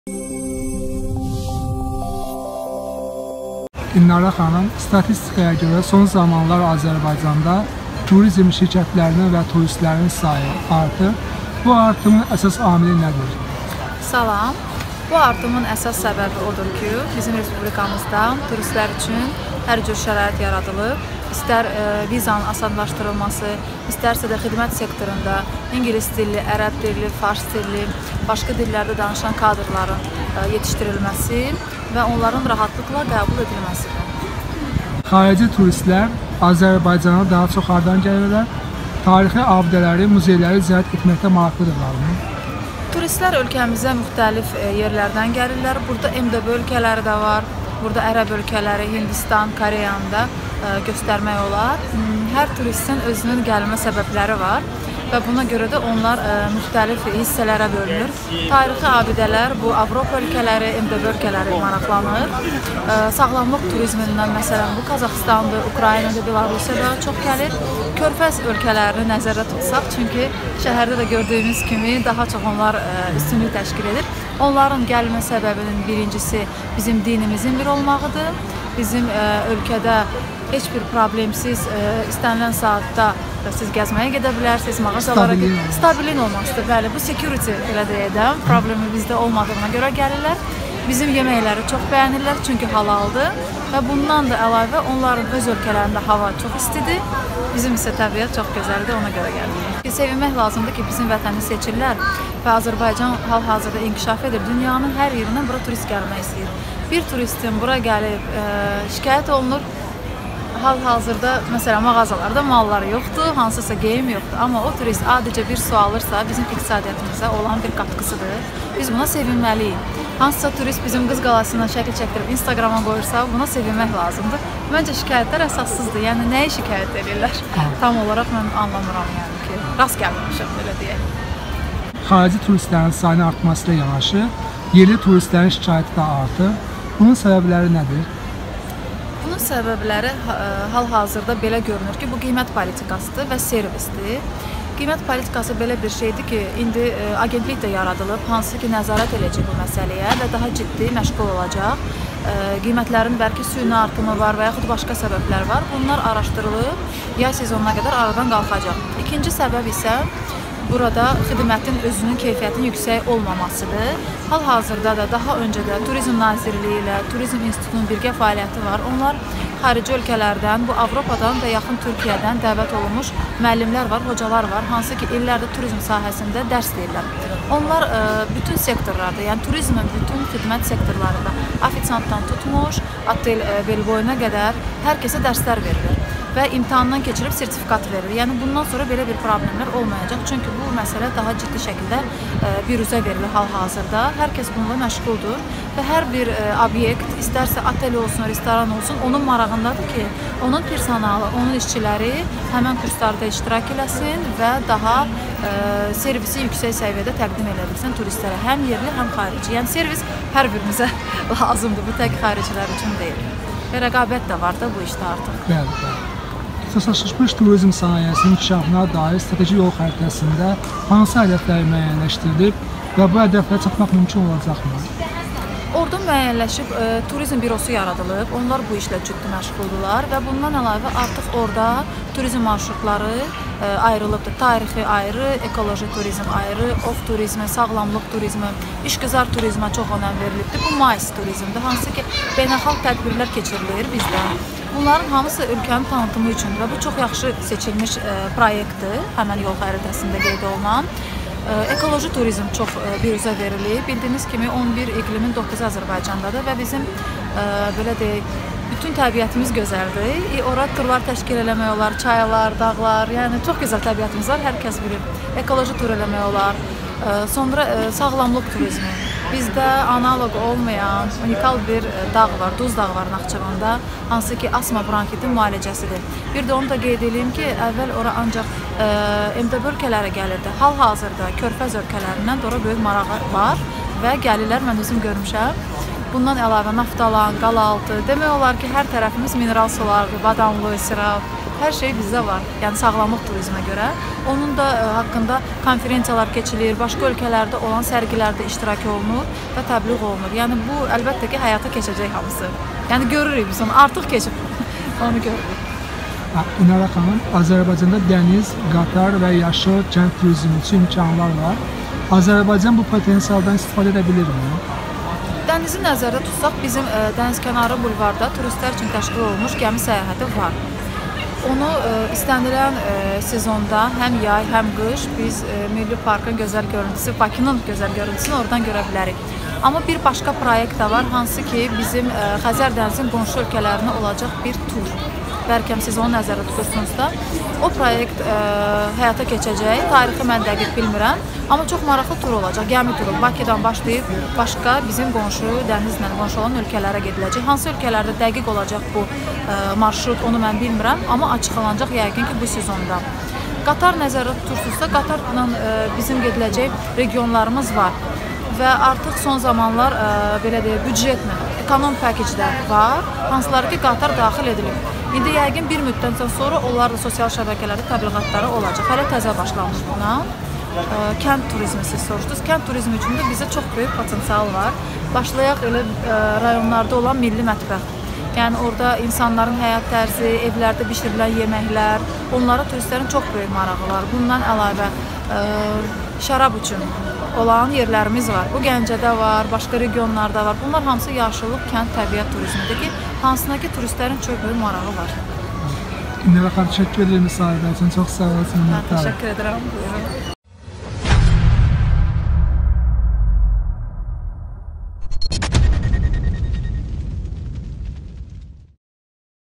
Niyə Avropalılar yox, ərəb turistləri Bakıya daha çox gəlir? QIZLARA görə?
Turizim üzr eksper İnarə İbrahimova ölkəmizə son zamanlar gələn turustlərin maraqları barədə danışıb. O, qeyd edib ki, turistlərin gəlmə səbəbləri müxtəlifdəir. Onlar tarixi abidələr, sağlamlıq və dun amilinə görə ölkəmizi seçirlər.